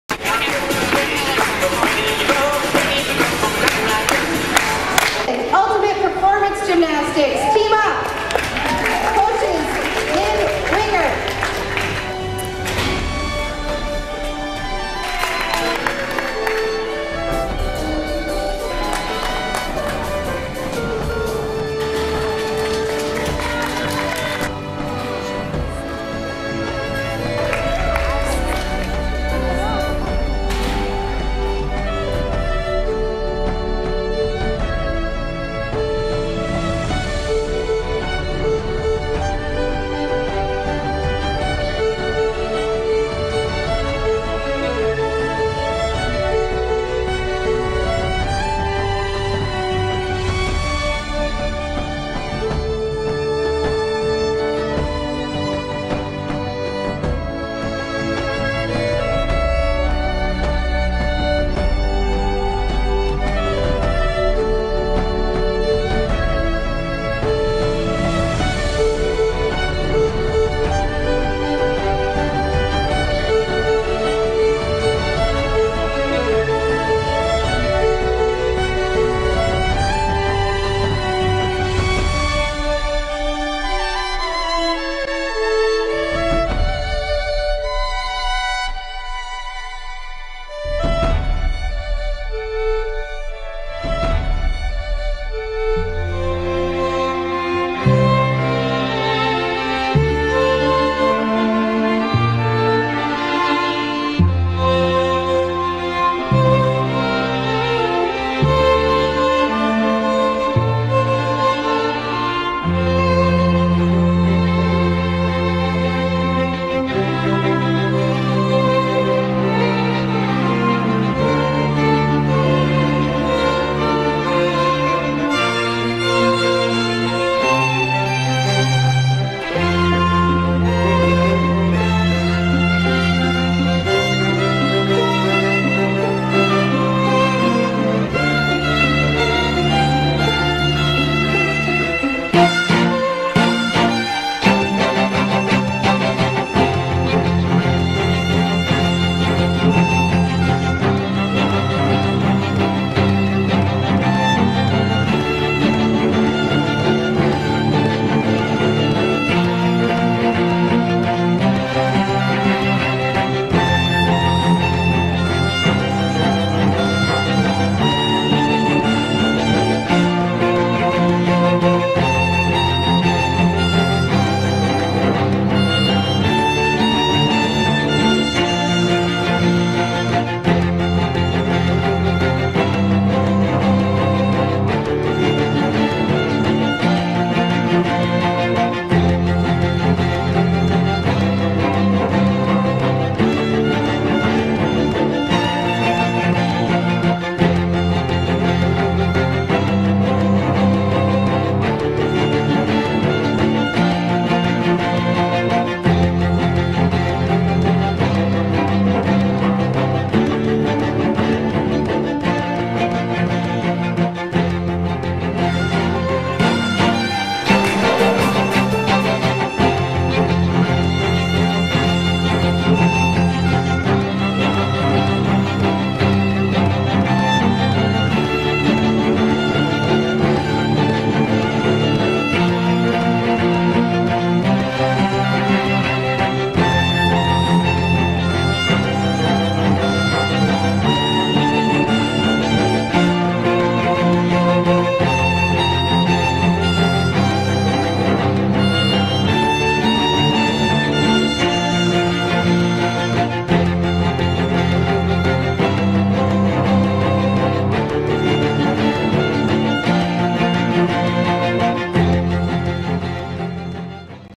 UPG Level 4's Competing at Flips Fall Hoedown!
Condensed video footage of UPG Level 4's competing at Flips Fall Hoedown in North St. Paul. The team won second place in its second USAG gymnastics meet!